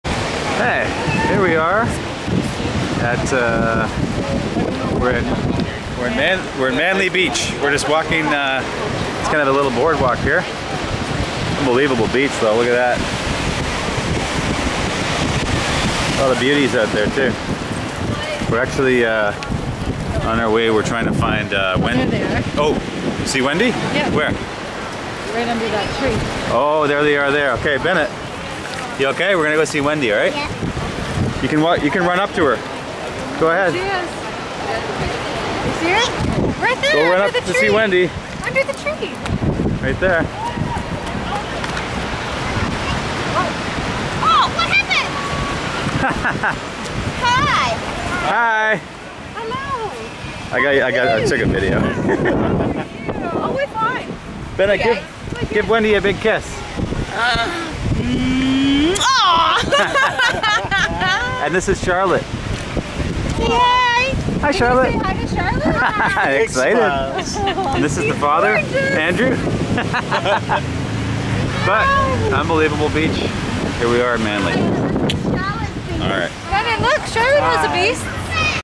Manly Beach.
Video of Manly Beach, meeting up with Wendy.